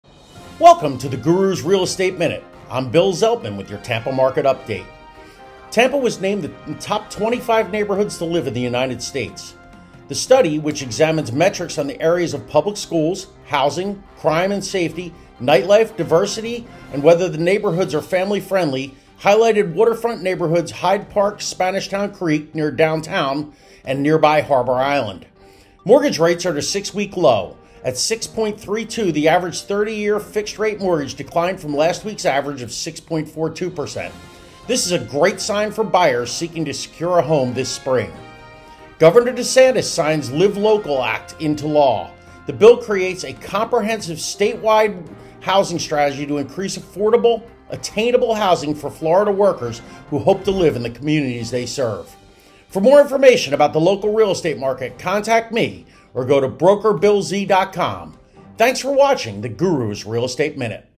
Gurus Real Estate Minute Tampa Bay Area Market Update 3 31 2023
Get your up to date weekly real estate market report from the Gurus Real Estate Minute - Tampa Bay Area Market Update 3/31/2023. For more information about the local real estate market, contact your Official Real Estate Guru Bill Zeltman at or go to BrokerBillZ.com.